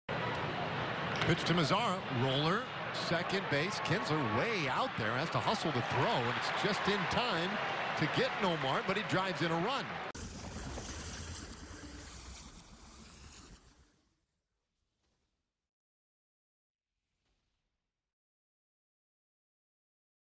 Nomar Mazara RBI groundout
Mazara scratches out the Rangers first run of the game on a chopper to the second baseman.